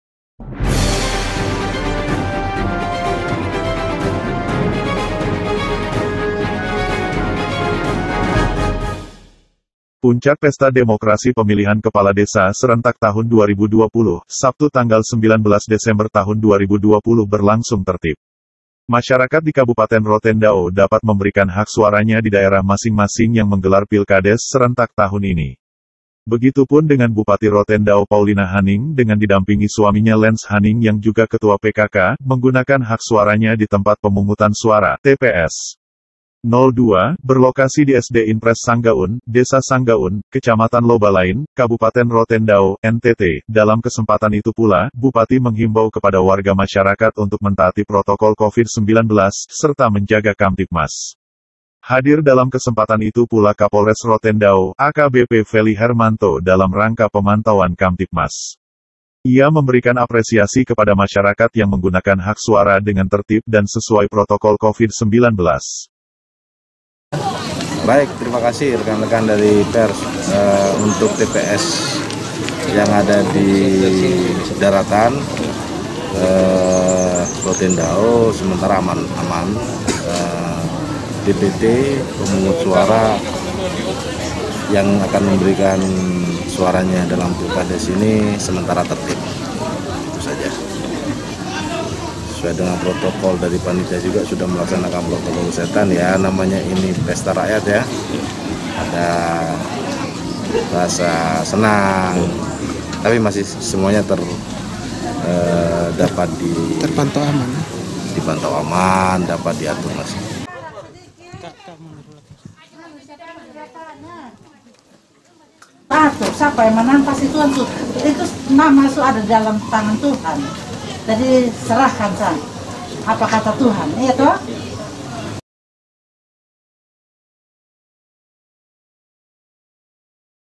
Hari Ini 69 Desa di Rote Ndao Laksanakan Pilkades Serentak
ROTETVNEWS.COM, ROTE : Puncak pesta demokrasi Pemilihan Kepala Desa   serentak tahun 2020, Sabtu  19 Desember 2020 berlangsung tertib.
 Masyarakat dapat memberikan hak suaranya di daerah masing-masing yang menggelar Pilkades serentak tahun ini.

Begitupun dengan Bupati Rote Ndao Paulina Haning dengan didampingi Suaminya Lens Haning yang juga Ketua PKK, menggunakan hak suaranya di Tempat Pemungutan Suara (TPS) 0 2, berlokasi di SD Inpres Sangga oen, Desa Sangga oen, Kecamatan Lobalain, Kabupaten Rote Ndao, NTT.
dalam kesempatan itu pula, Bupati menghimbau kepada warga masyarakat untuk mentaati protokol covid 19, serta menjaga kamtibmas.

Hadir dalam kesempatan itu pula Kapolres Rote Ndao, AKBP Felly Hermanto dalam rangka pemantauan kamtibmas.
ia memberikan apresiasi kepada masyarakat yang menggunakan hak suara dengan tertib dan sesuai protokol covid 19.
[21:05, 12/18/2020] Jerry: Puncak pesta demokrasi Pemilihan Kepala Desa   serentak tahun 2020, Sabtu  19 Desember 2020 berlangsung tertib.
 Masyarakat Di kabupaten Rote Ndao dapat memberikan hak suaranya di daerah masing-masing yang menggelar Pilkades serentak tahun ini.

ia memberikan apresiasi kepada masyarakat yang menggunakan hak suara dengan tertib dan sesuai protokol covid 19. (RO-01)